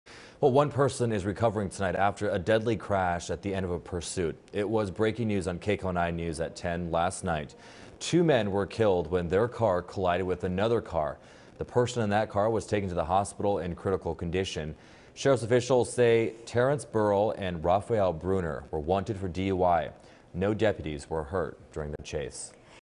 Willowbrook Pursuit Ends With 2 Fatalities, 1 Critically Injured on Saturday Evening
Two men involved in a pursuit with Los Angeles County Sheriff's Deputies were killed on Saturday evening at the tail end of the chase, when they collided with a traffic signal. No deputies were injured in the incident, however one person unrelated to the pursuit was recovering on Sunday.